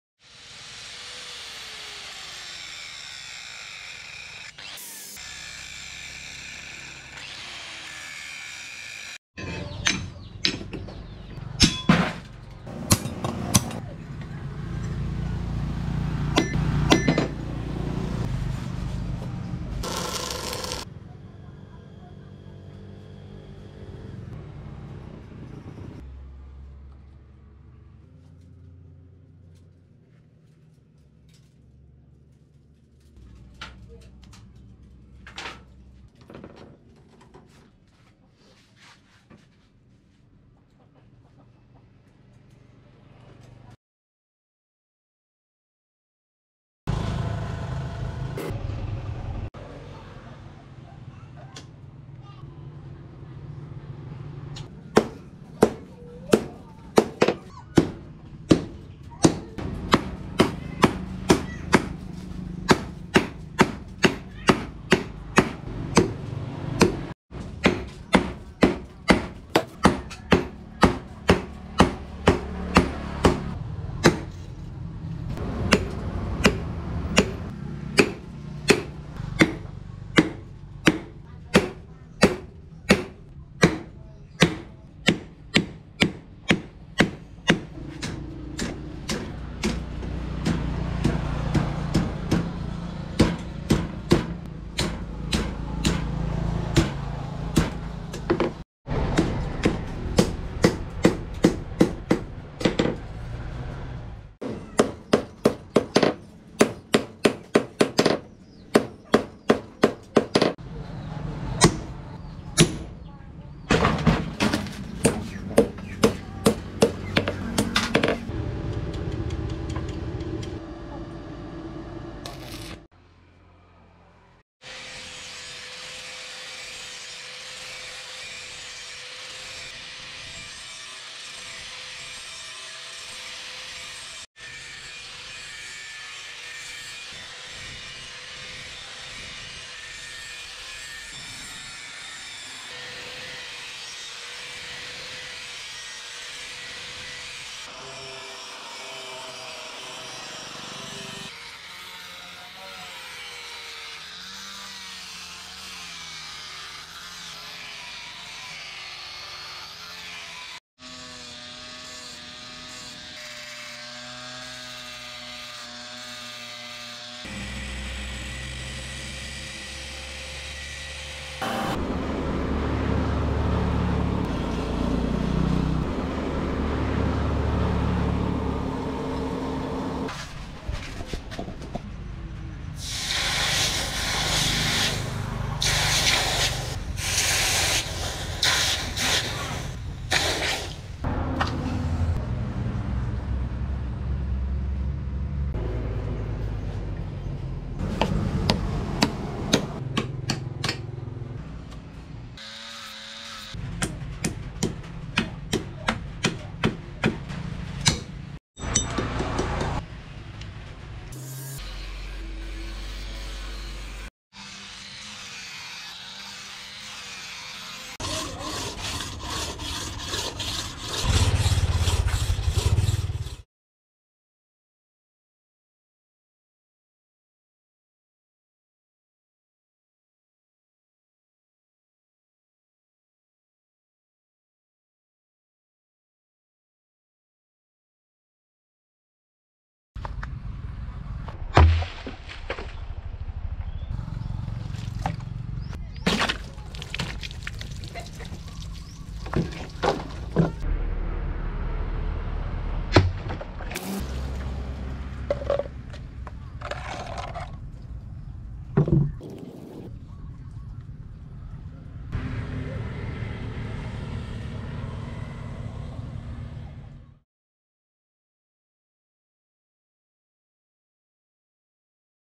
Forging a TWIST REBAR DAGGER.! For Fun!
Blacksmith forging a twist rebar dagger for fun!

Hope you enjoyed and like!

Best Wishes,